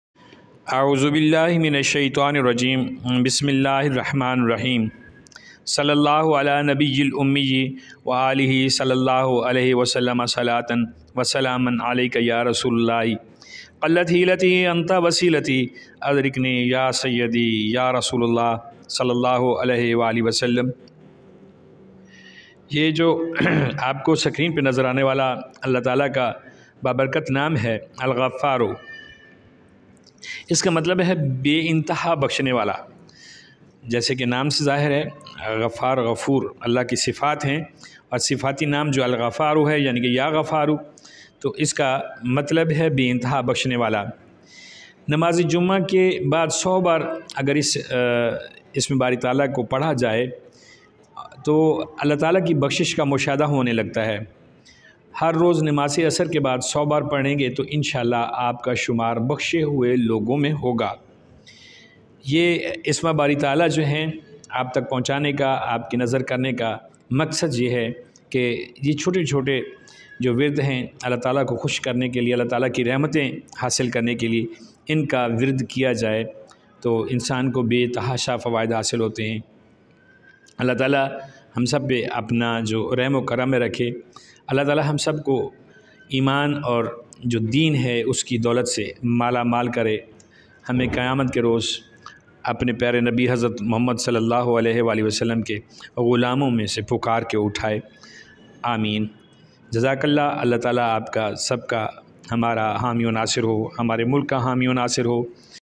ALLAH BLESSINGS | AL-GHAFFAR'O@ms21newshd
اس اسم کو جمعہ کی نماز کے بعد سو بار پڑھیں تو الله تعالٰی کی بخشش کا مشاہدہ ہونے لگے گا۔ ہر روز نمازِ عصر کے بعد سو بار پڑھیں گے تو انشاء الله آپ کا شمار بخشے ہوئے لوگوں میں ہوگا@ms21newshd